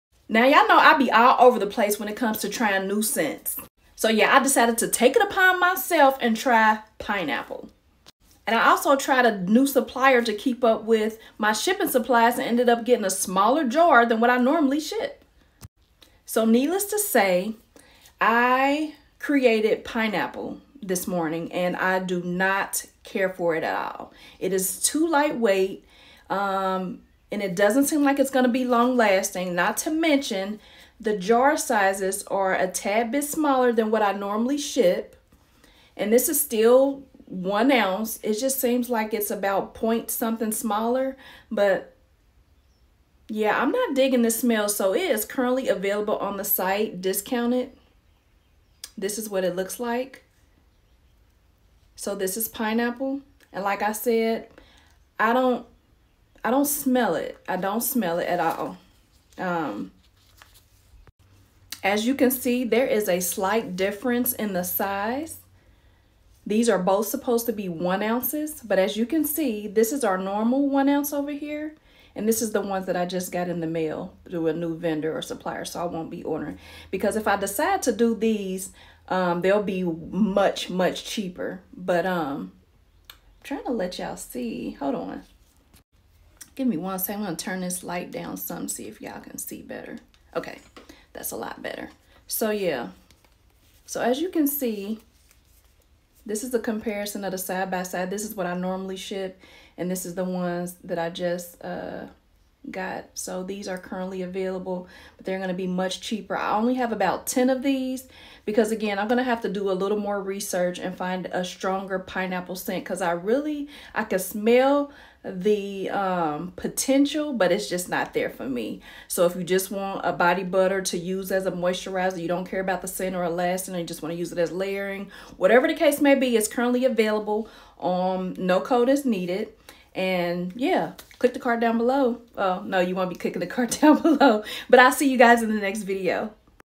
LIGHTLY scented Pineapple Triple Whipped Body Butter
🌟 Whipped Body Butter Alert! 🌟 Dive into a tropical paradise with our 1oz Pineapple Scented Body Butter Moisturizer! 🍍✨ This little jar packs a punch of hydration, leaving your skin feeling as smooth as a piña colada. 🏝️ Whether you’re on vacay or just dreaming of one, this pineapple-infused delight is your passport to glowy, radiant skin. 🌺🧴